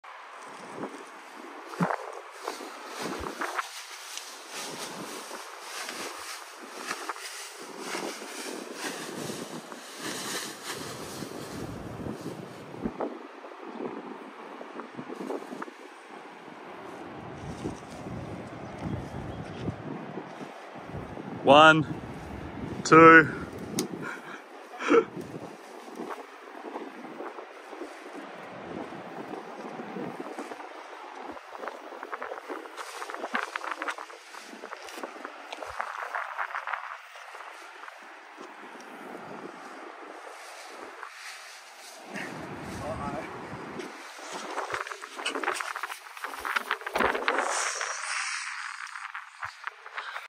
Top landing on a handrail
Top landing my Flare Moustache Parakite at Sunshine Beach, QLD, Australia.
Flaring is similar to Paragliding, but completely different. It’s a new way of flying, opening up different opportunities.

If you have any questions regarding the Flare Moustache get in touch. I’m happy to chat.
I’m 75 kg, flying the Flare Moustache 18m. 15-17 knots, ridge height 40-50m

Manufactory Apparel - Paragliding and Extreme Sports Jersey